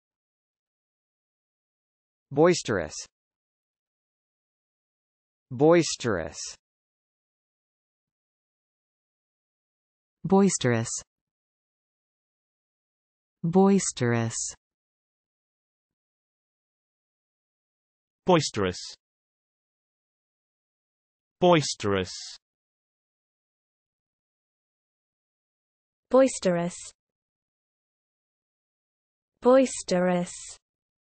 How to pronounce boisterous
Learn how to say Boisterous in English correctly with TextToSpeech.io free pronunciation tutorials. American & British English pronunciation of male & female voices provided in videos. Check them out!

    Boisterous definitions:
    (s): violently agitated and turbulent; "boisterous winds and waves"; "the fierce thunders roar me their music"- Ezra Pound; "rough weather"; "rough seas"
    (s): noisy and lacking in restraint or discipline; "a boisterous crowd"; "a social gathering that became rambunctious and out of hand"; "a robustious group of teenagers"; "beneath the rumbustious surface of his paintings is sympathy for the vulnerability of or
    (s): full of rough and exuberant animal spirits; "boisterous practical jokes"; "knockabout comedy"